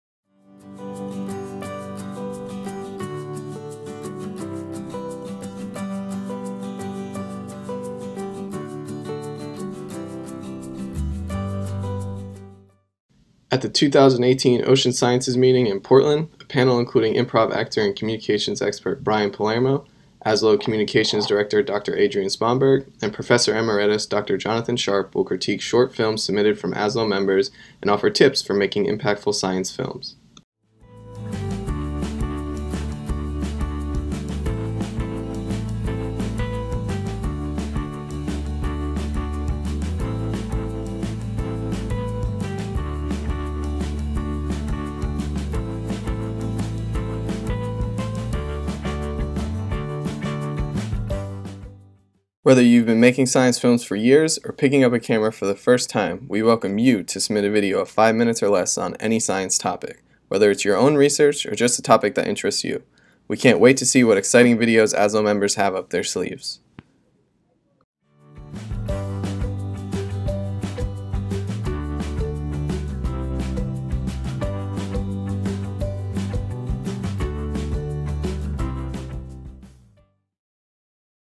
2018 OSM: Science Videos that Engage
Science Videos that Engage
2018 Ocean Sciences Meeting, Portland OR
Video Workshop at the 2018 Ocean Sciences Meeting
Tuesday February 13th
7:30 – 9:30 PM
Room B117 – 119

Join us for a workshop and  learn how to make short videos that  capture attention and are compelling  to a broad “public” audience through commentary led by Hollywood insider, Brian Palermo

We invite anyone interested to submit a short video, not to exceed 5 minutes in length in YouTube format before the meeting in February. Submissions are encouraged from all Ocean Sciences attendees, including: a broad array of graduate students, and early career scientists

This years workshop we will screen several participant-submitted science videos and provide commentary with an eye towards critiquing and creating the most effective narrative and connection with audience through the submitted videos.

The workshop is designed to help you make effective, engaging and compelling videos and will be led by Brian Palermo, professional actor and improv instructor, Adrienne Sponberg, ASLO director of communications, and professor emeritus Jonathan Sharp.

This effort is partially supported by a grant from the Ocean Sciences Division of the US National Science Foundation.